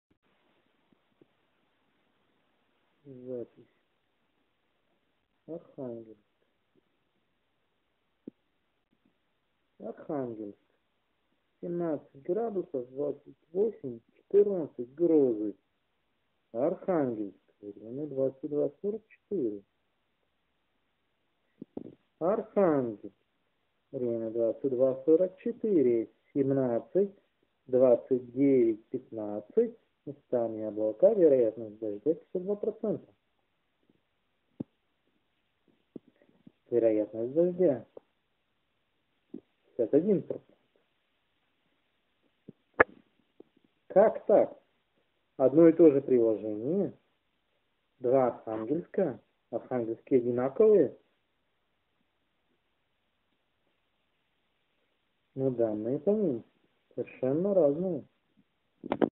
Прикол с отображением погоды в Архангельске на смартфоне
Прикол с отображением погоды в городе Архангельске
в программе AccuWeather
1. Архангельск - по GPS координатам
2. Архангельск - выставлен принудительно